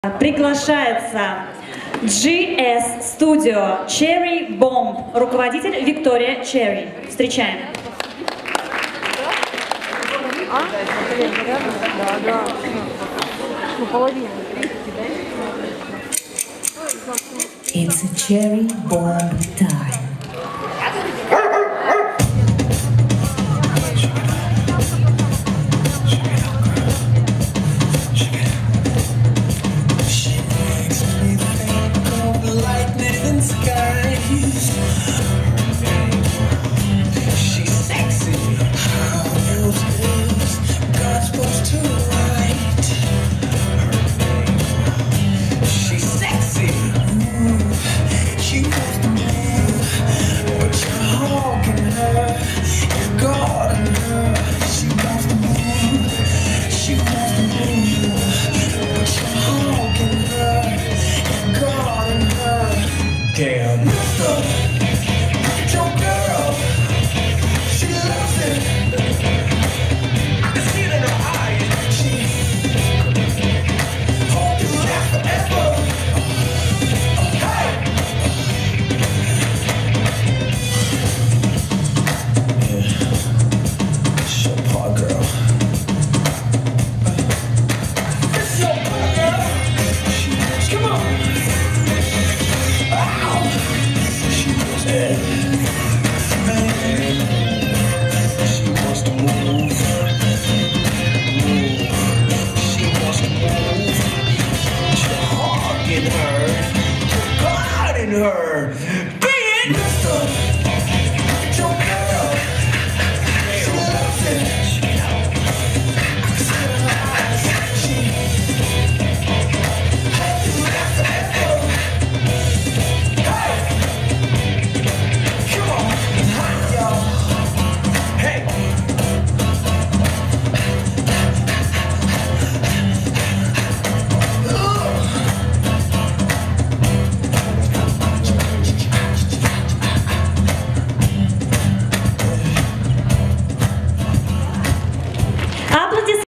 GS studio, Cherry Bomb - Dance Star Festival - X 24.04.16.
Dance Star Festival - X 24.04.16.
2 часть фестиваля.
Любители и Профи взрослые группы.
Все детские номинации.

Монтаж:
Tardis Arts
PHOTO VIDEO AUDIO